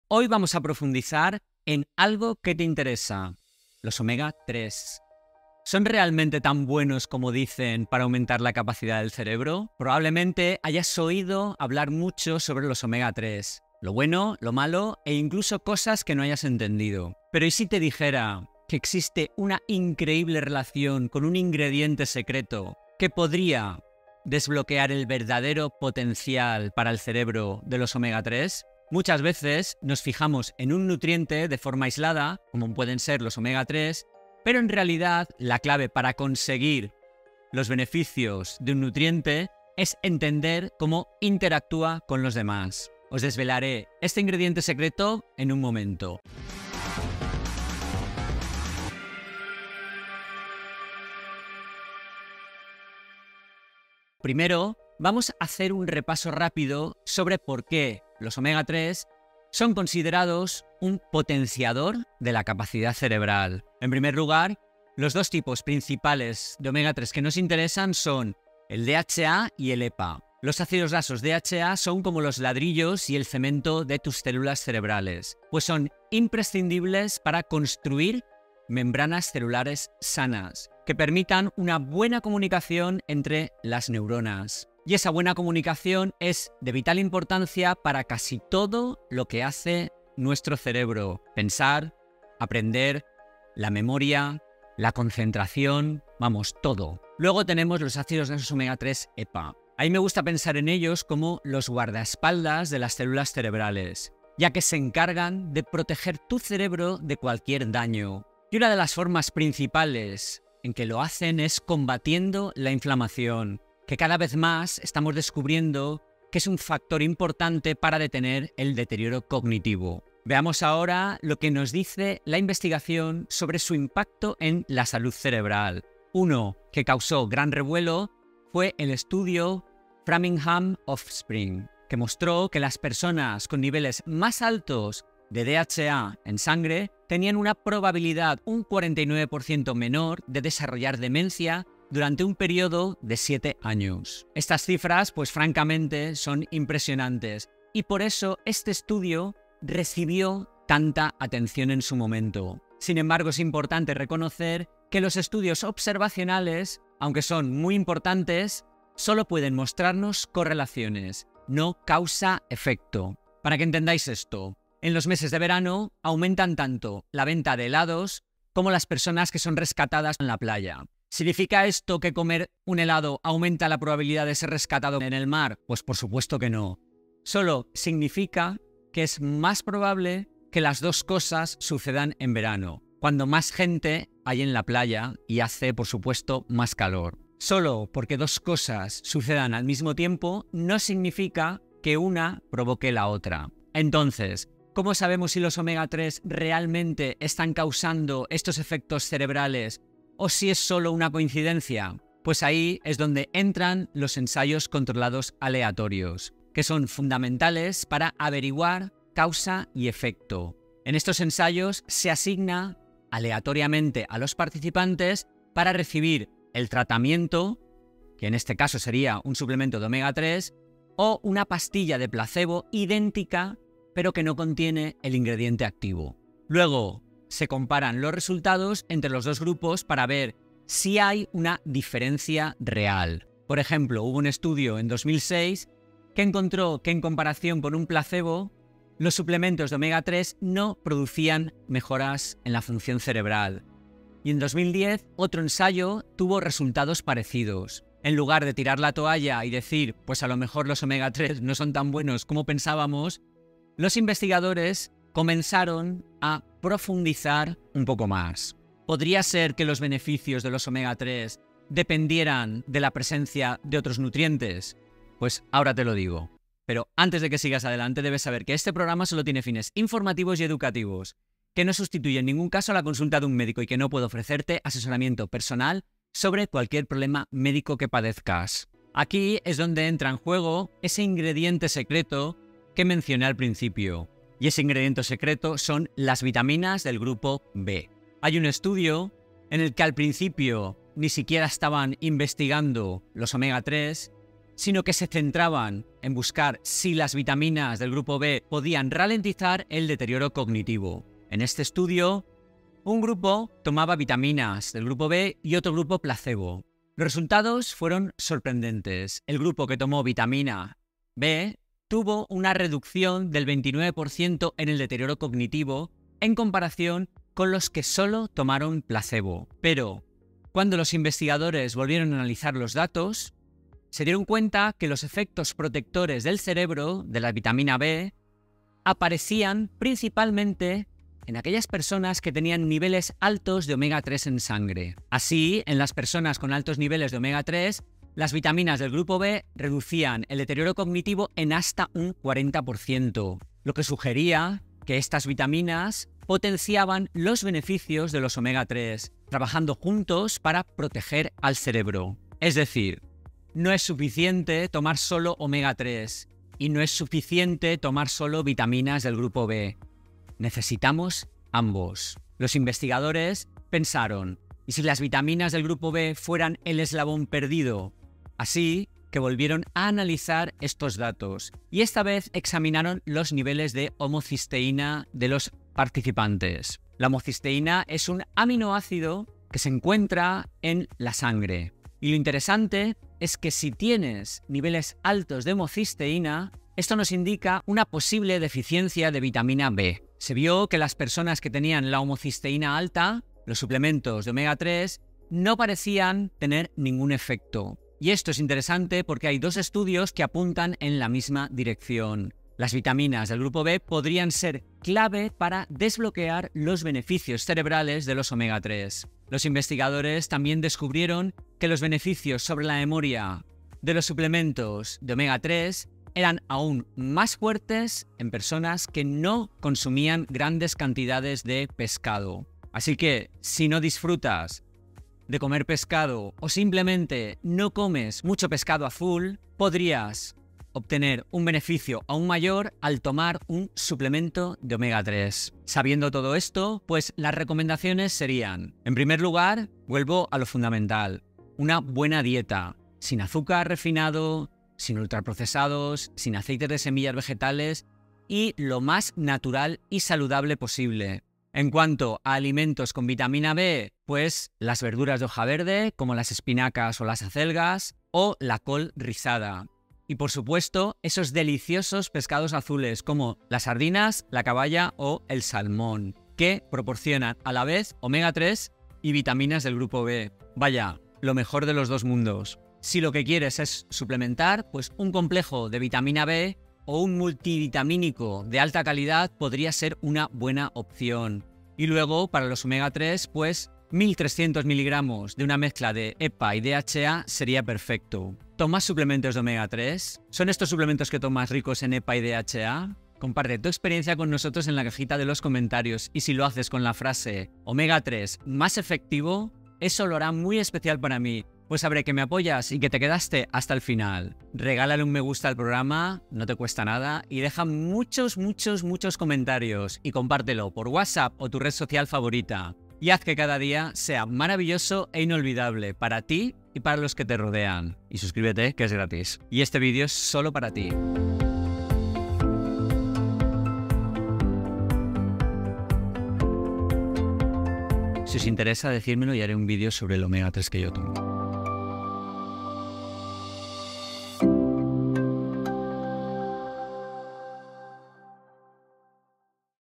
OMEGA-3 en tu CEREBRO un DESCUBRIMIENTO tentador
Un hallazgo innovador está transformando nuestra comprensión sobre el omega-3 y su impacto en la salud del cerebro. Existe un factor clave que faltaba, el cual aclara por qué en algunos estudios se observan mejoras significativas en la memoria y las capacidades cognitivas, mientras que en otras ocasiones el omega-3 no parece tener efecto.

00:00 - Introducción: OMEGA-3 en tu CEREBRO un DESCUBRIMIENTO tentador
01:03 - DHA y EPA, ¿qué hacen por tu cerebro?
02:00 - omega 3 y el riesgo de demencia
04:12 - ¿Qué hace la ciencia para demostrar su efectividad?
05:01 - ¡El ingrediente secreto que potencia los omega 3!
07:15 - Cómo obtener omega 3 y vitaminas B de forma natural

Descubre:

· Cómo los ácidos grasos DHA y EPA fortalecen y protegen tus células cerebrales.
· El papel esencial de las vitaminas B en potenciar los efectos de los omega-3.
· Estudios científicos que respaldan esta poderosa combinación.
· Alimentos clave para incluir en tu dieta y maximizar estos beneficios.

Comparte tu experiencia: ¿Tomas suplementos de omega-3? ¿Son ricos en EPA y DHA? Deja tu comentario con la frase “OMEGA-3 EFECTIVO” para saber que te quedaste hasta el final y apoyas nuestro canal. 🙌

Bibliografía

1. Smith AD, Smith SM, de Jager CA, Whitbread P, Johnston C, Agacinski G, et al. Homocysteine-lowering by B vitamins slows the rate of accelerated brain atrophy in mild cognitive impairment: a randomized controlled trial. PLoS One [Internet]. 2010;5(9):e12244.

2. Sala-Vila A, Satizabal CL, Tintle N, Melo van Lent D, Vasan RS, Beiser AS, et al. Red blood cell DHA is inversely associated with risk of incident Alzheimer’s disease and all-cause dementia: Framingham offspring study. Nutrients [Internet]. 2022 [citado el 23 de septiembre de 2024];14(12):2408.

3. Zhang Y, Chen J, Qiu J, Li Y, Wang J, Jiao J. Intakes of fish and polyunsaturated fatty acids and mild-to-severe cognitive impairment risks: a dose-response meta-analysis of 21 cohort studies. Am J Clin Nutr [Internet]. 2016;103(2):330–40.

4. Quinn JF, Raman R, Thomas RG, Yurko-Mauro K, Nelson EB, Van Dyck C, et al. Docosahexaenoic acid supplementation and cognitive decline in Alzheimer disease: A randomized trial. JAMA [Internet]. 2010 [citado el 23 de septiembre de 2024];304(17):1903.

5. Chew EY, Clemons TE, Agrón E, Launer LJ, Grodstein F, Bernstein PS, et al. Effect of omega-3 fatty acids, lutein/zeaxanthin, or other nutrient supplementation on cognitive function: The AREDS2 randomized clinical trial. JAMA [Internet]. 2015;314(8):791–801.

6. El esstudio Framingham [Internet]. Sen.es. [citado el 23 de septiembre de 2024].

Información legal.
Vídeo de grabación propia.